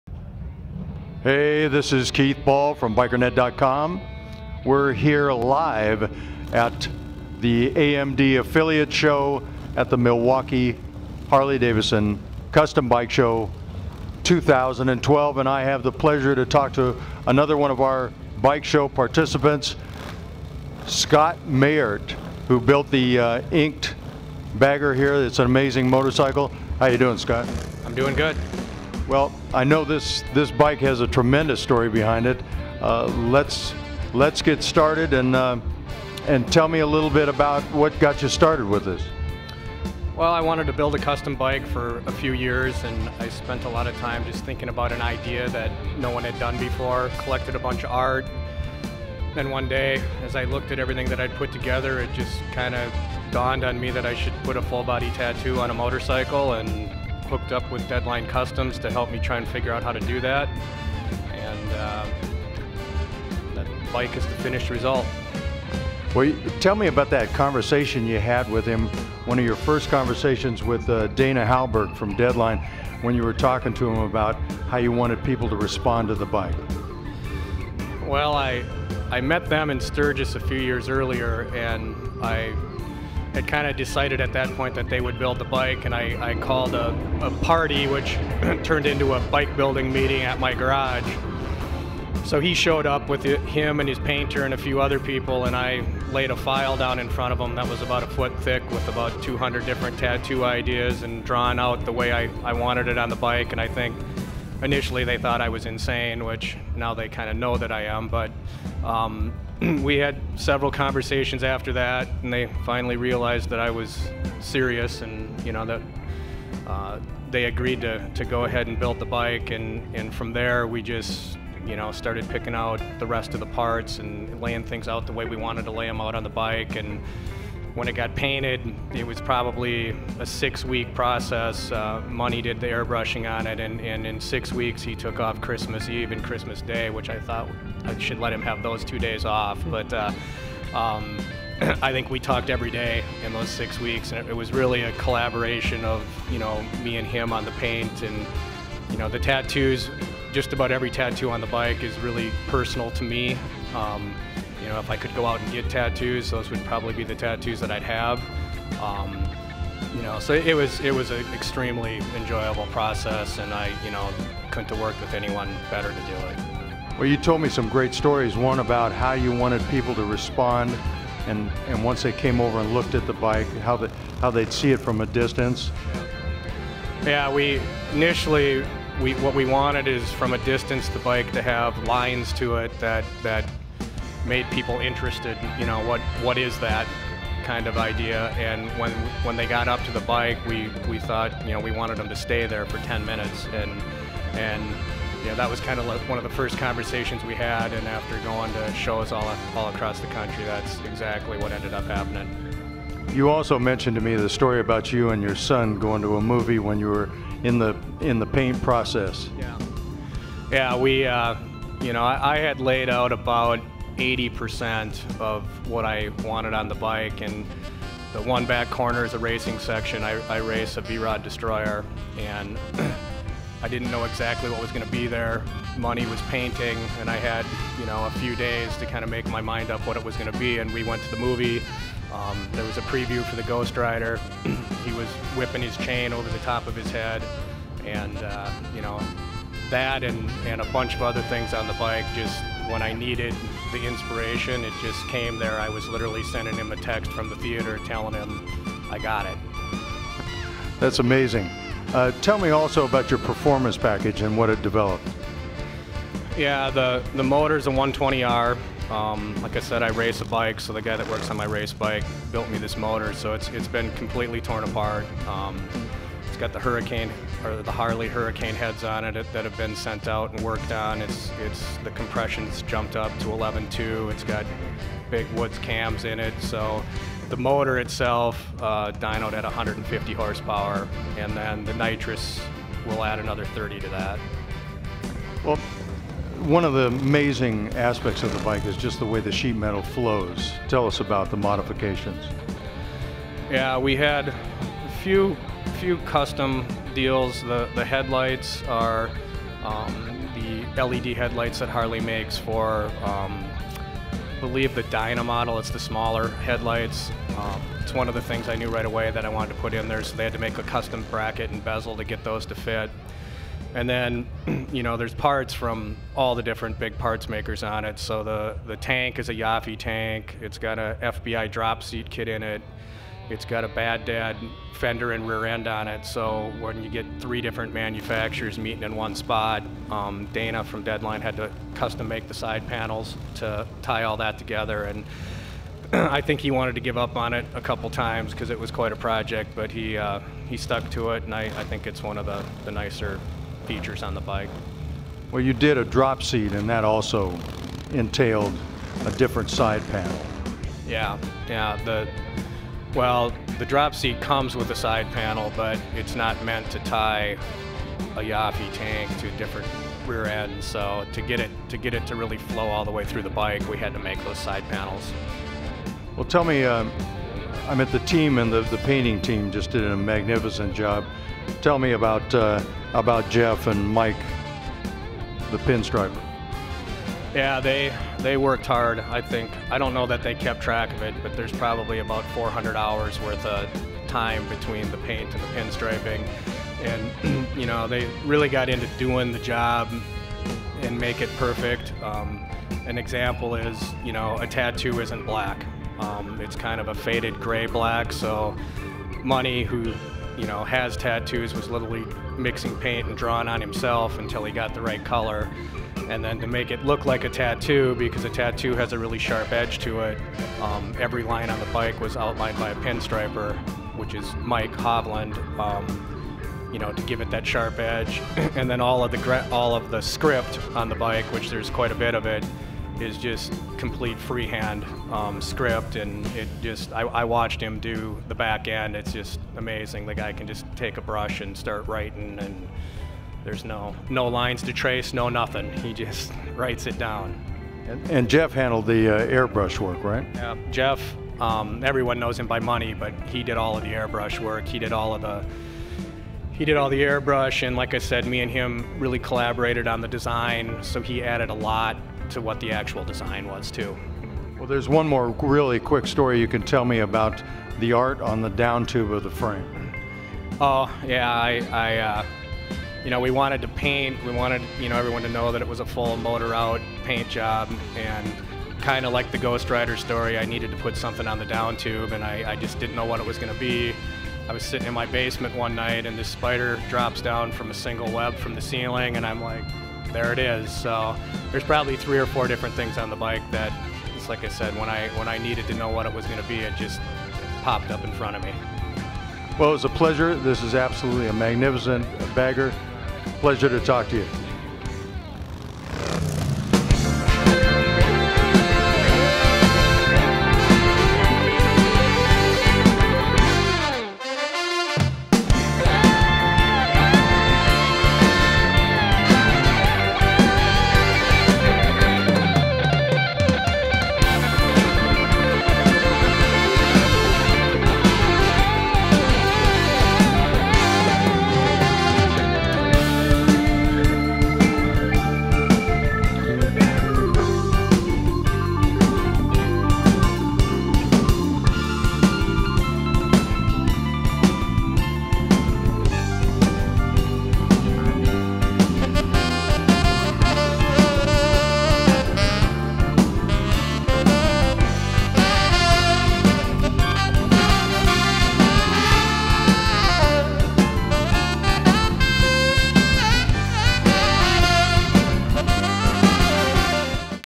Bikernet.com's Bandit interviews Scott Maeyaert about INK @ 2012 AMD Affiliate Show
"I want to be the first to do this" is the credo of all Custom Builders each time they start a new project. Dana Hallberg, the creative genius behind Deadline Customs, is always pushing the envelope with his designs. His latest build, INK, is a collaberation between Scott Maeyaert and Dana. It is the tattoo bike that has received so much attention. And each tattoo design has a story. Keith Ball of interviews Scott at the AMD Affiliate ProShow about the sled.